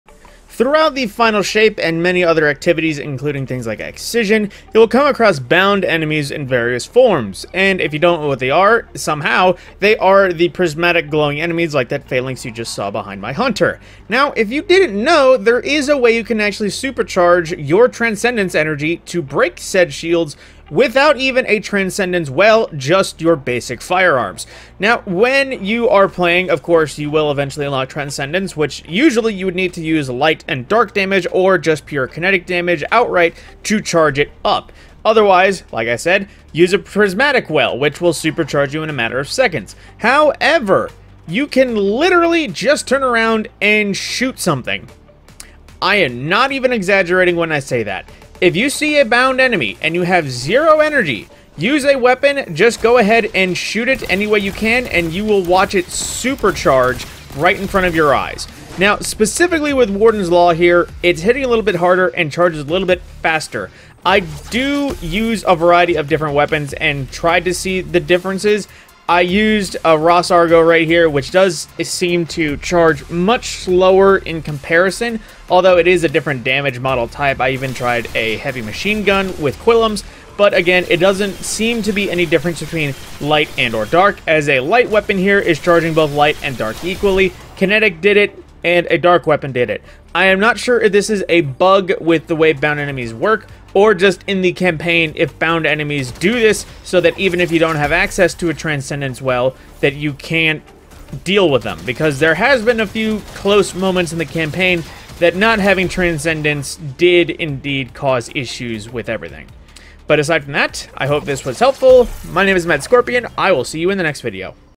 Charge Transcendence In SECONDS Without A Prismatic Well
In practice, transcendence can take a little while to charge and can be a pain with bound enemies. But Bungie either didn't tell or didn't know you can charge transcendence with basic small caliber rounds.